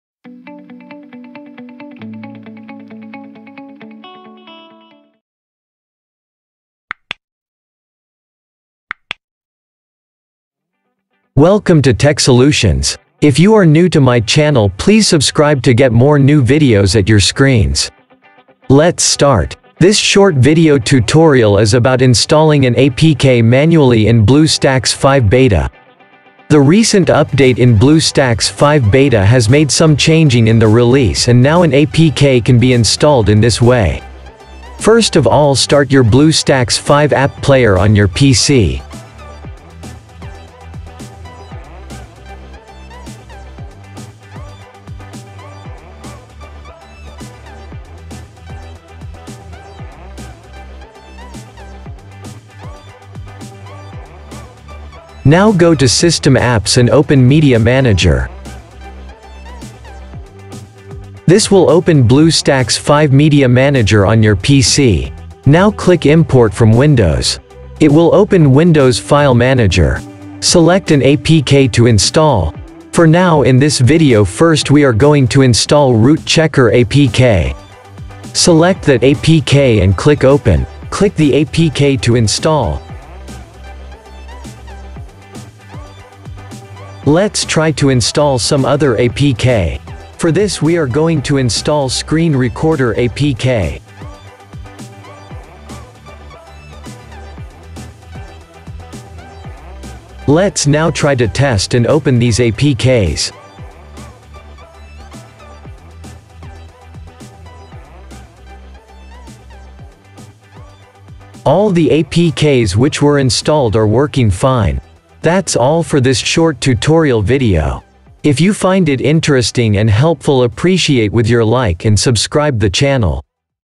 How to Install APK in Bluestacks 5 Beta 2021 | Bluestacks 5 Media Manager
Learn How to install APK in Bluestacks 5 Beta Player 2021. APK Install method in New Bluestacks 5 Android Emulator. This Video Will Guide you How you can Install an APK File in Bluestacks 5 Beta with New Media Manager. In the New Media Manager APKs can be Imported from Windows to Bluestacks 5 Installation. Then an APK can be Installed in This way as shown in the Video Guide.
Download Bluestacks 5 from official Bluestacks.com